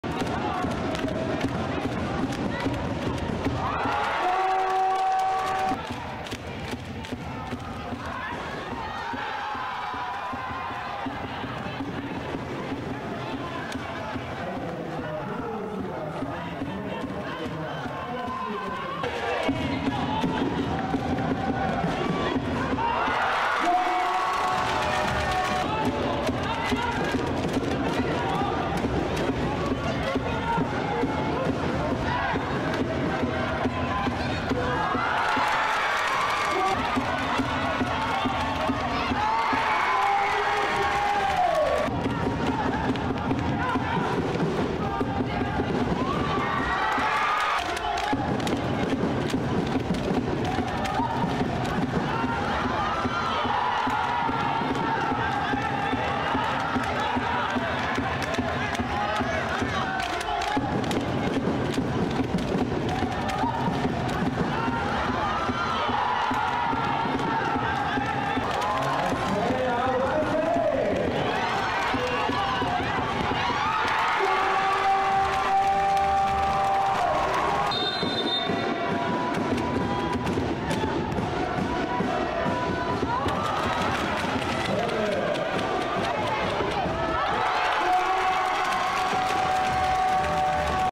【Ｆリーグ2023-2024 ディビジョン1 第17節】フウガドールすみだ 2-7 バサジィ大分
F.LEAGUE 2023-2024 Division 1 Sec.17
FUGADOR SUMIDA 2-7 VASAGY OITA
日本フットサルリーグ(Ｆリーグ)所属
フウガドールすみだ